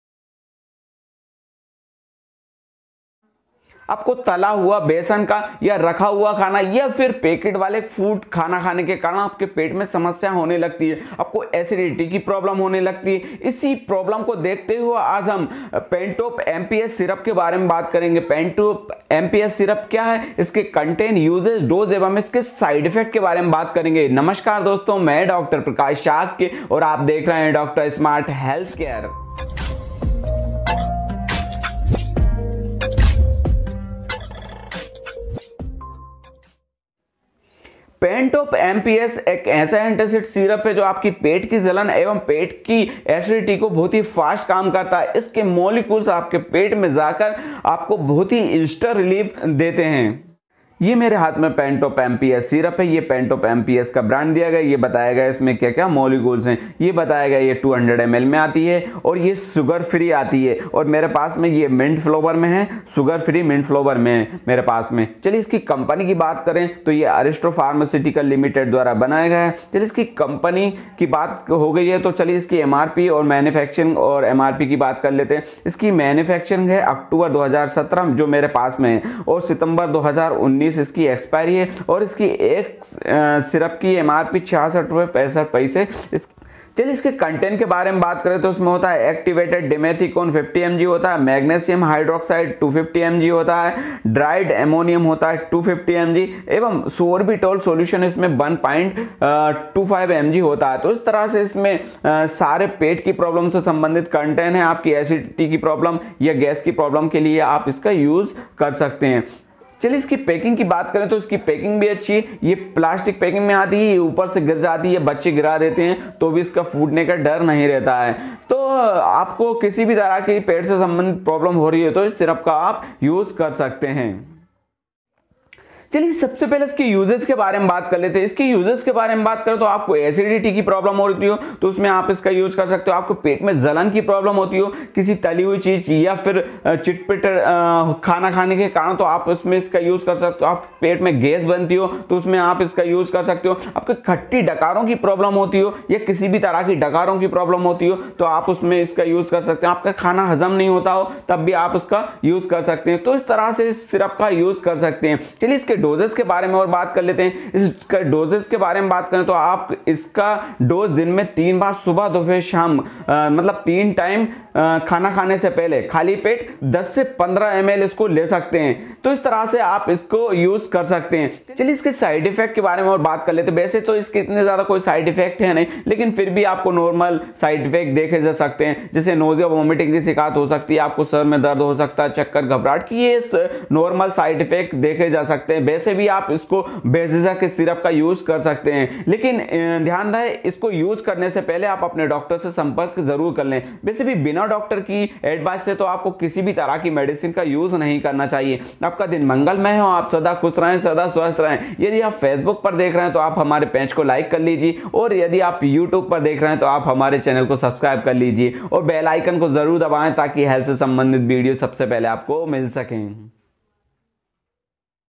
Pantop MPS syrup uses, dose and side effect
pantop MPS syrup uses dose and side effect ke baare mein video ke dwara Jankari di Gayi Hai iss syrup ka use Karke app acidity ki problem ko door kar sakte hai